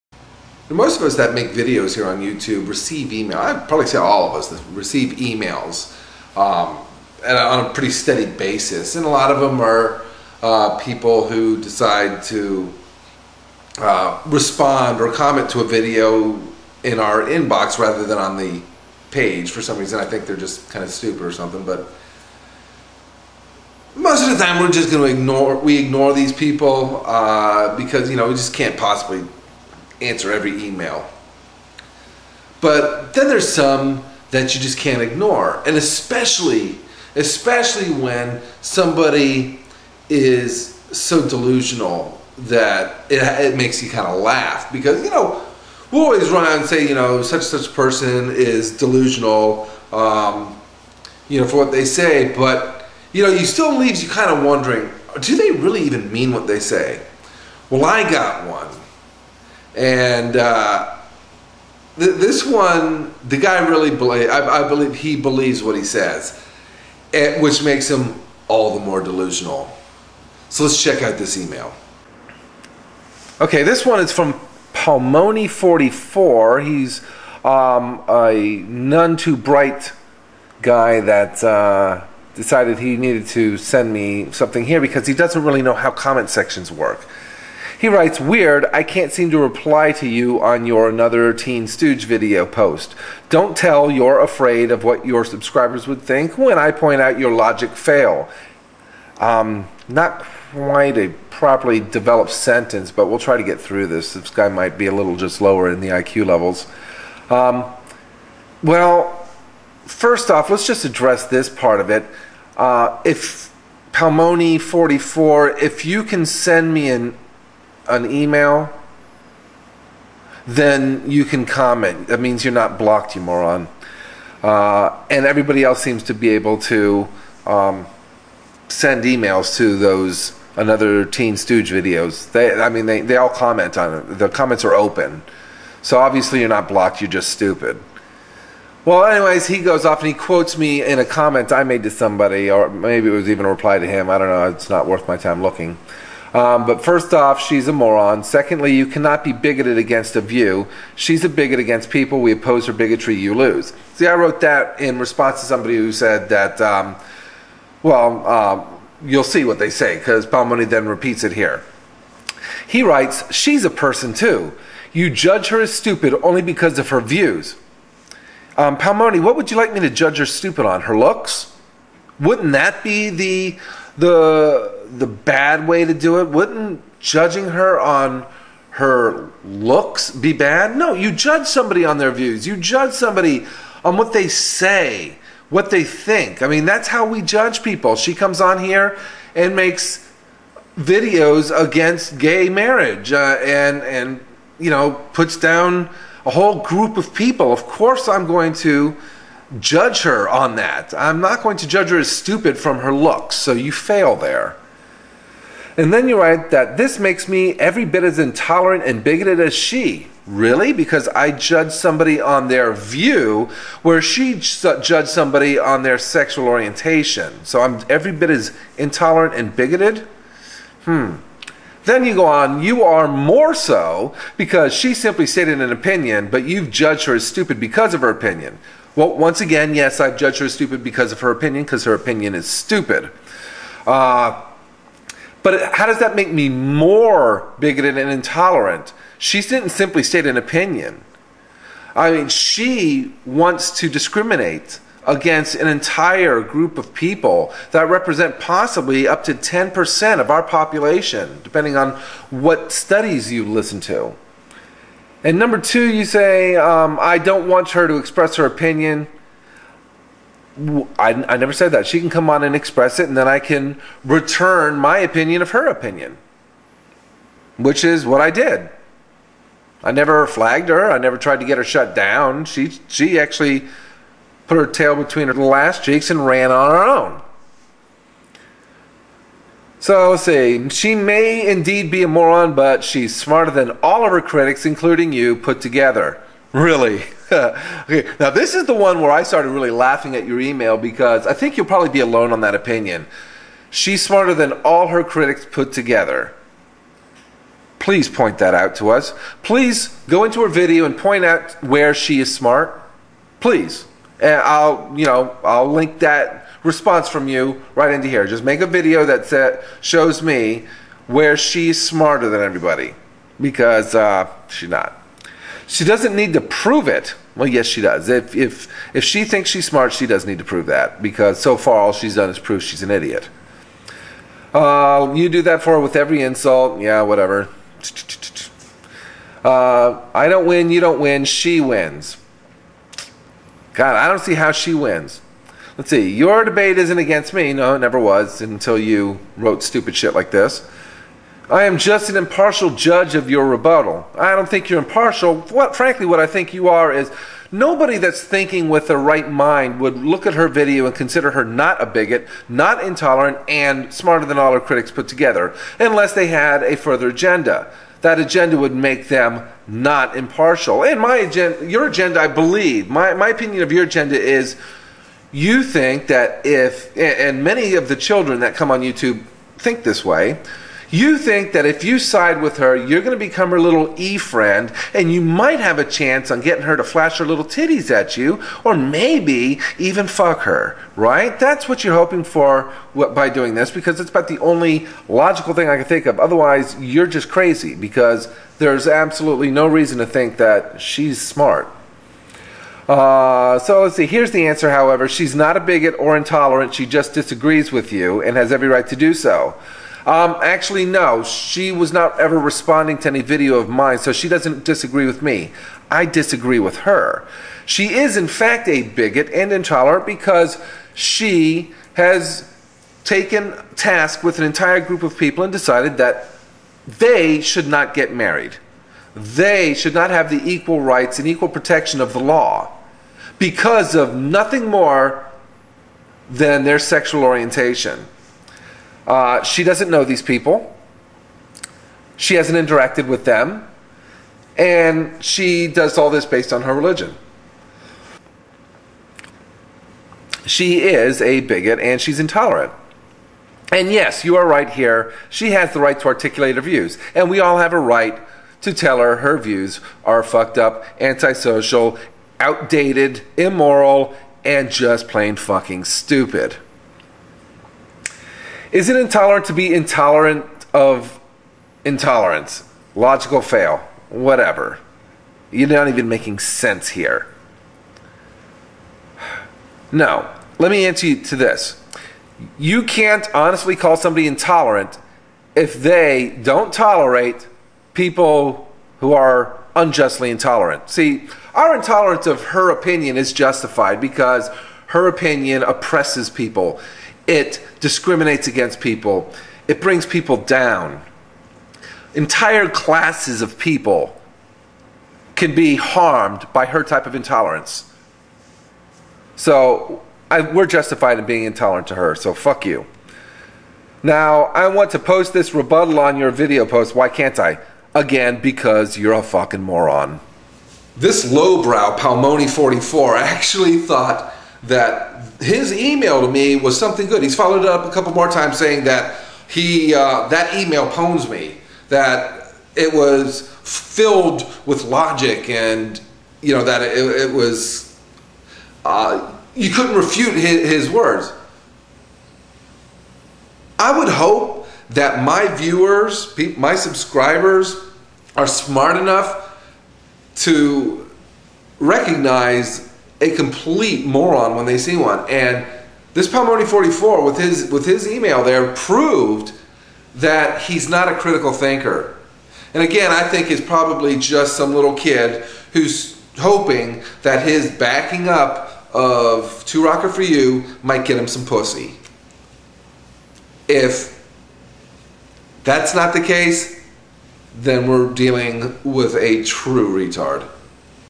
Palmoni44: When Cretins Come Out To Play
Taking an email and using it to pwn its sender.